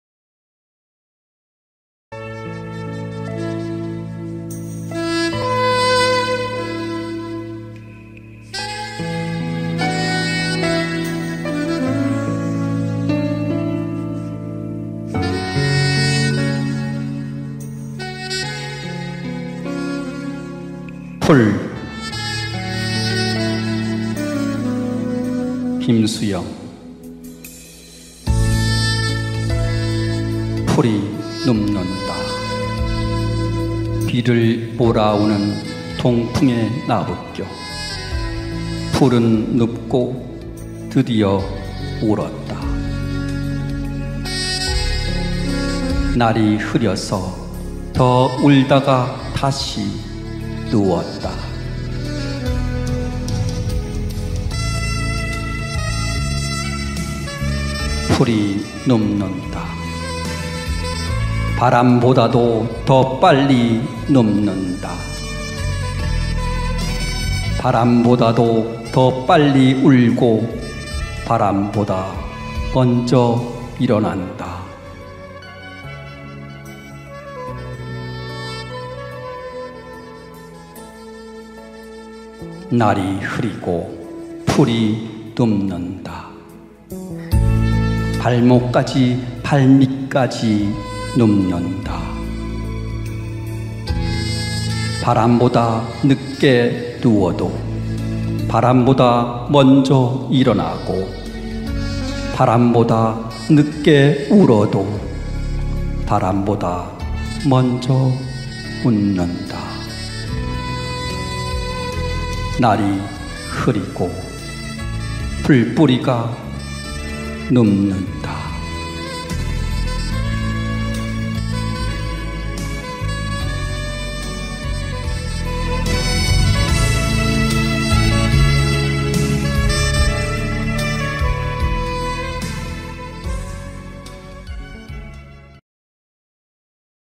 홍영길 - 풀 [시 - 김수영] [ 서수옥의 감성힐링 詩 낭송 톡 콘서트 ]
2020년 8월 17일

[ 서수옥의 감성힐링 詩 낭송 톡 콘서트 ]

장    소:문톡TV 스튜디오 촬영
총 감 독: 임희종
촬영, 편집 : 한스영상
음   향 : 임기수
기   획 : 유경미
기획, 사진, 배경 문의  - 유경미 공일공 육삼공하나 일이이공